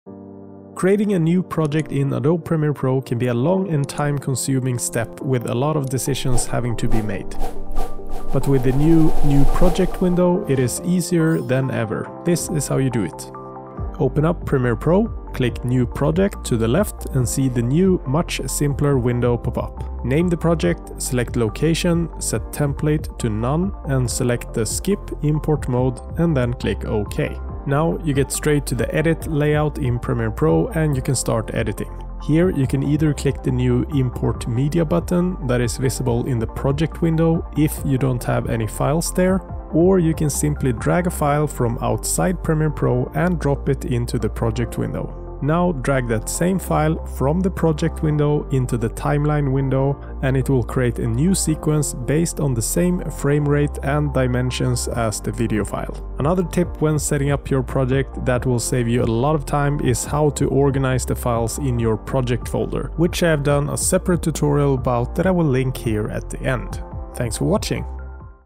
Easiest way to start a project in Adobe Premiere Pro
Creating a new project in Adobe Premiere Pro can be a long and time-consuming step with a lot of decisions having to be made.
But with the new “new project” window, it is easier than ever.

This is how you do it:
Open up Premiere Pro. Click “New project” to the left and see this new much simpler window pop up.
Name the project, select location, set template to “None” and select the “Skip import mode” and then click “OK”.

Now, you get straight to the “Edit” layout in Premiere Pro and can start editing. Here you can either click the new “Import media”-button that is visible in the “Project” window IF you don’t have any files there, or you can simply drag a file from outside Premiere Pro and drop it into the “Project” window.

(The “import media”-button will disappear after importing any footage, but you can still open the import window by clicking ”Cmd + I” on Mac or "Ctrl + I" on PC. You can also import through the menu by going to "File" and "Import".)

Now drag that file from the “Project” window into the “Timeline” window and it will create a new sequence based on the same framerate and dimensions as the file.

Another tip when setting up your project that will save you a lot of time is how to organize the files in your project folder, which I’ve done a separate tutorial about that I will link here at the end.